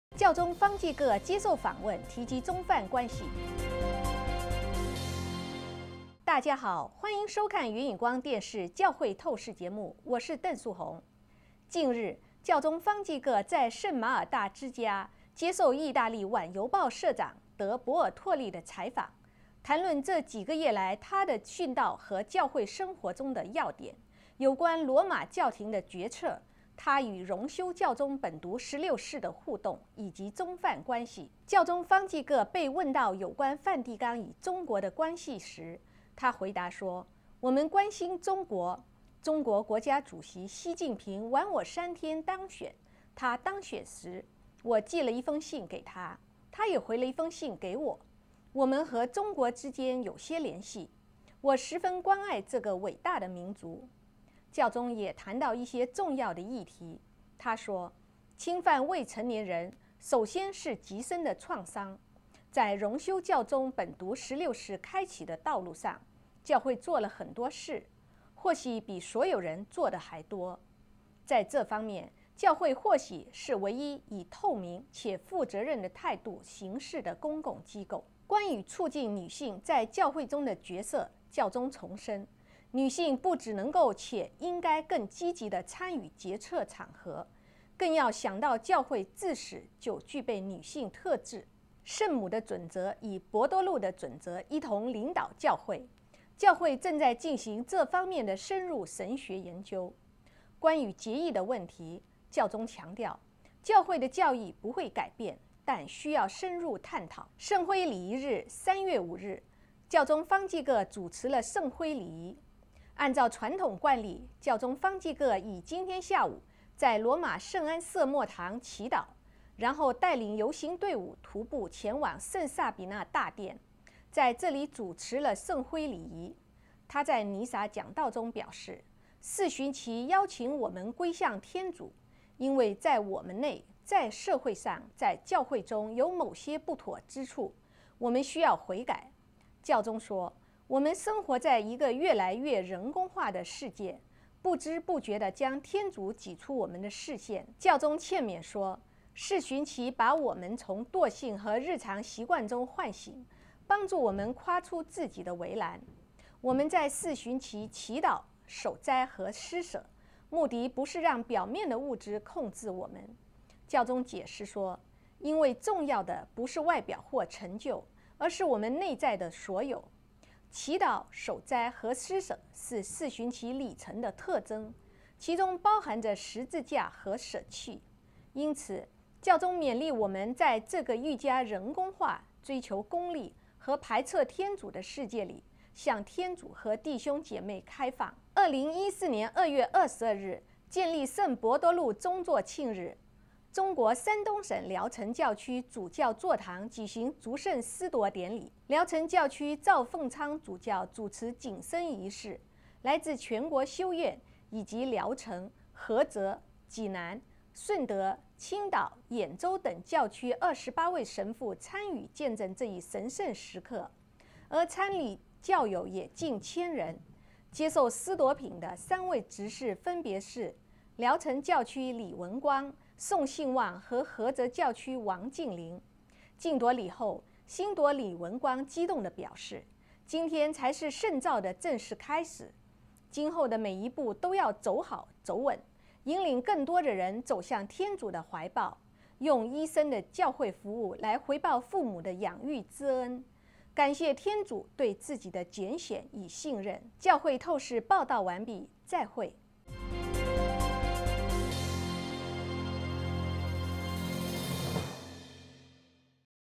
教会透视 Perspectives -教宗方济各接受访问提及中梵关系 - 国语 Mandarin
-教宗方济各接受访问提及中梵关系
-教宗方济各主持圣灰礼仪
-中国山东省聊城教区主教座堂举行祝圣司铎典礼